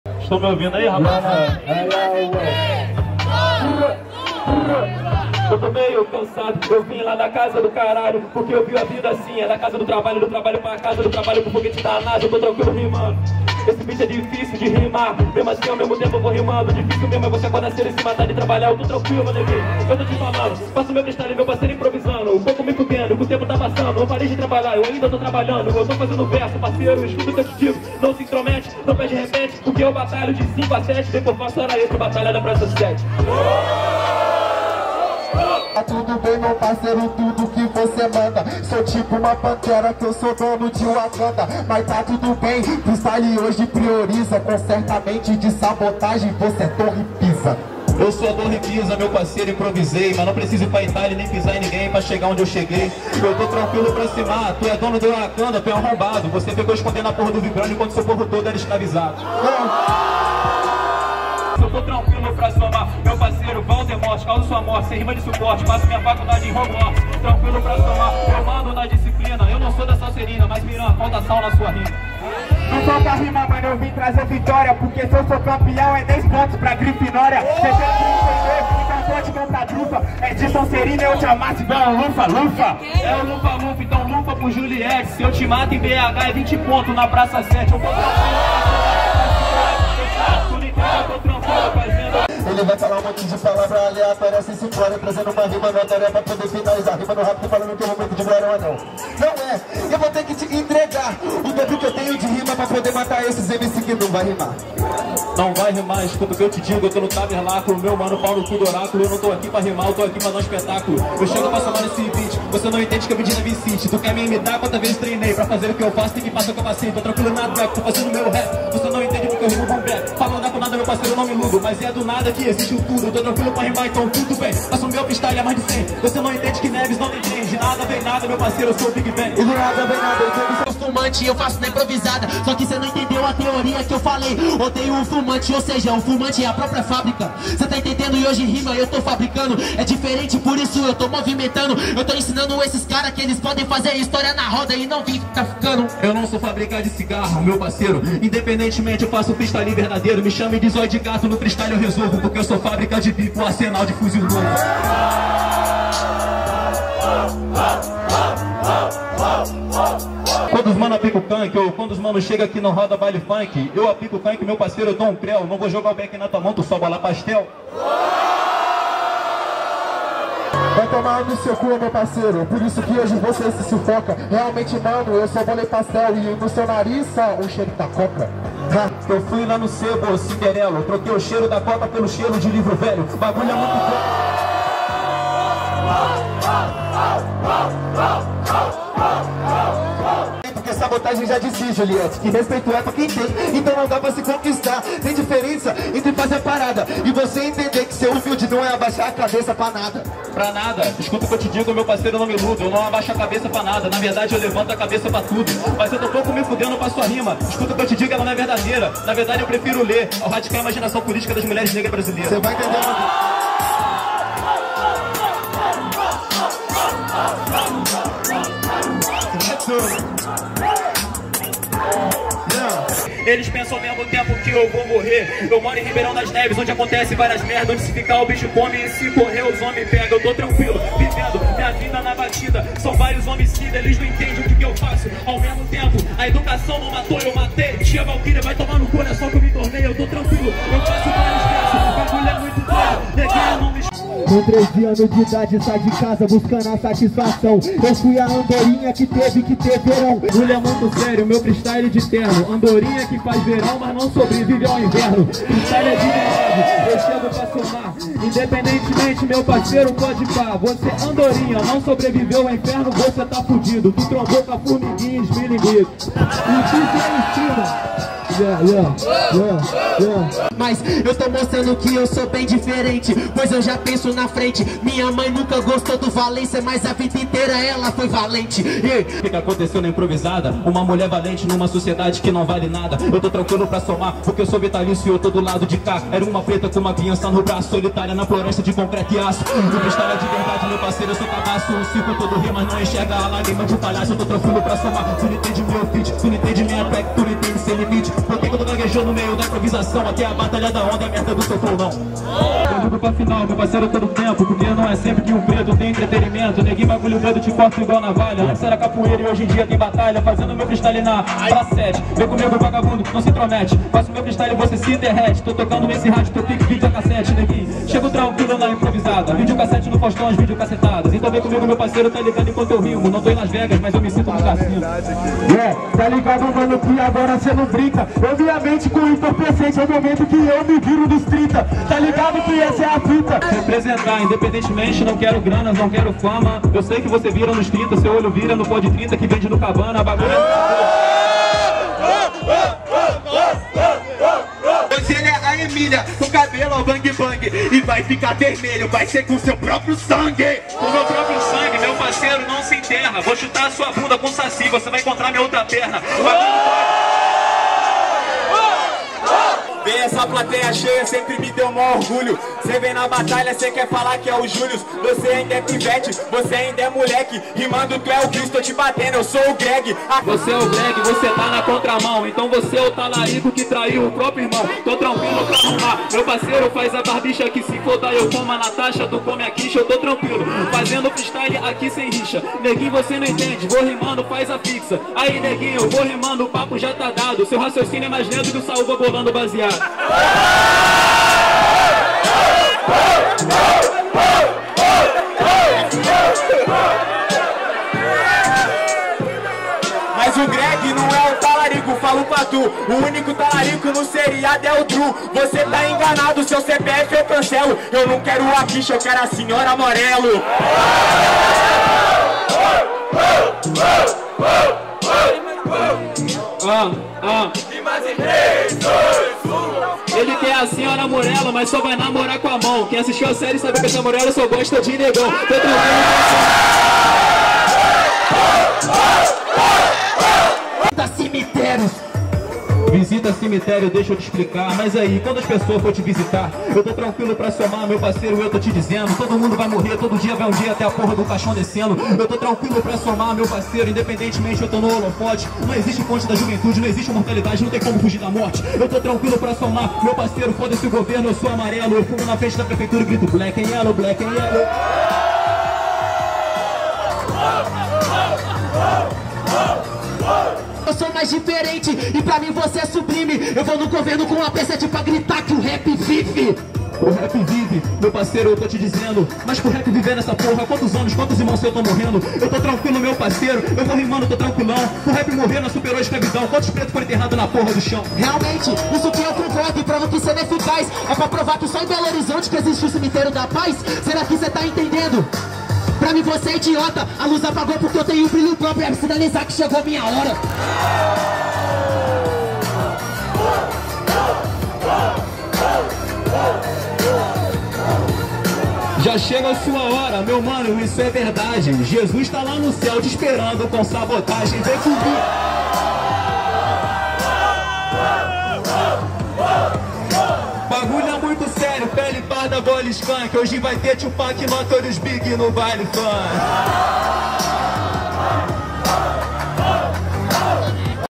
JULIET | CAMPEÃO DO RANKING | BP7 - 1/2 2024
BP7 Toda quinta: 19:30 na Praça 7 (BH É QUEM?)

Produção: D_7 @d7__d7
Mestre de Cerimônia: SAUVA @sauva_mc
Direção de Fotografia: RAISSA MAIA @raissa.maia_
Edição: GUSTAVO ANDRADE @guhcandrade
Assistente de Edição: MAXADO @maxadomc